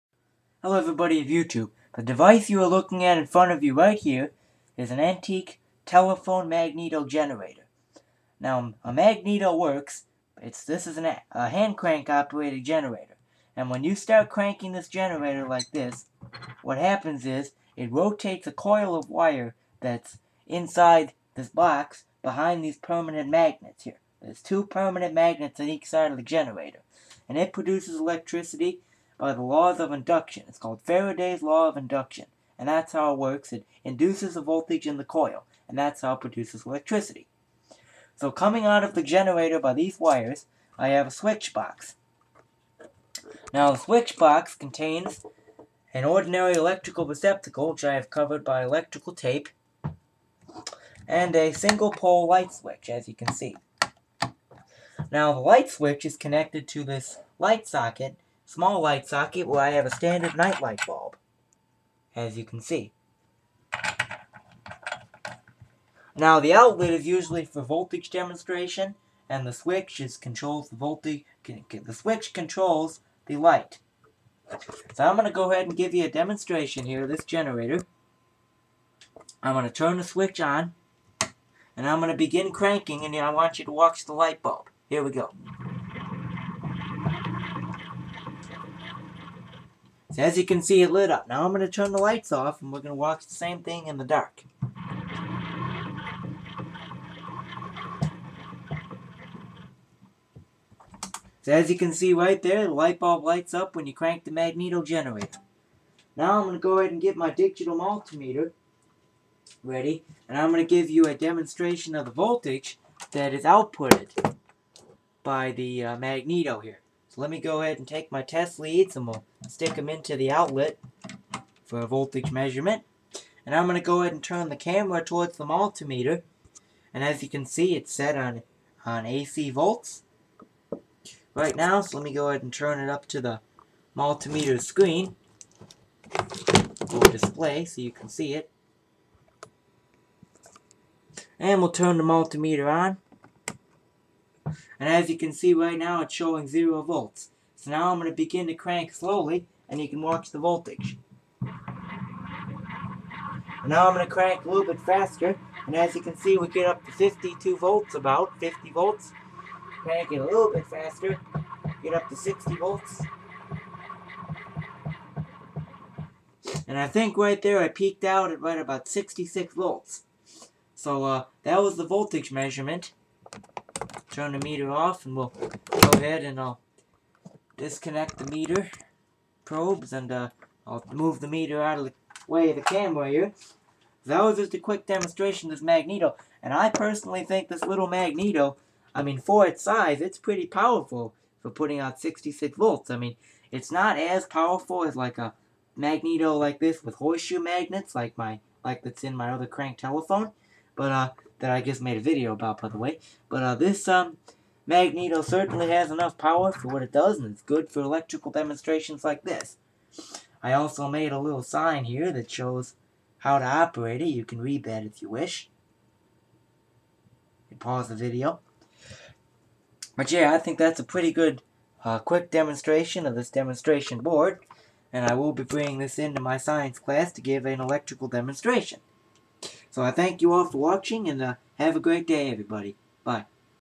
Hand Crank Magneto Generator Demonstration Board
This is a short video about my demonstration board using a telephone magneto for science class.